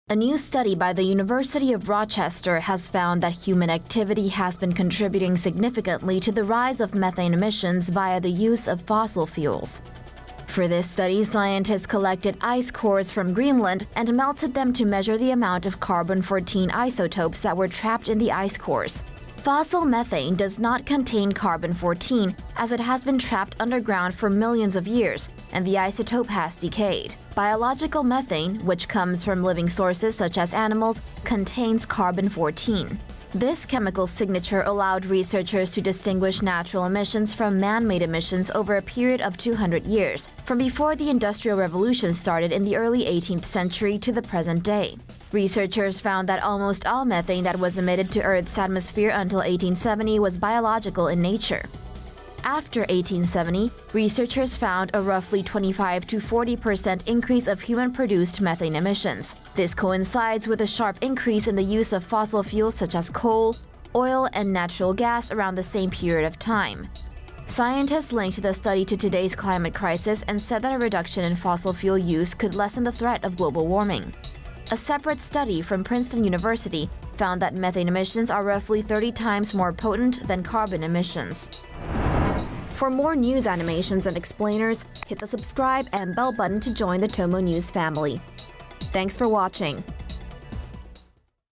Increased methane emissions linked to fossil fuel usage - TomoNews
ROCHESTER, NEW YORK — A new study published by the University of Rochester in Nature magazine has found that human activity has been contributing significantly to the rise of methane emissions via the use of fossil fuels.

SOURCES: Technology Review, Nature Magazine, Science Daily via University of Rochester, Science Daily via Princeton University, The Verge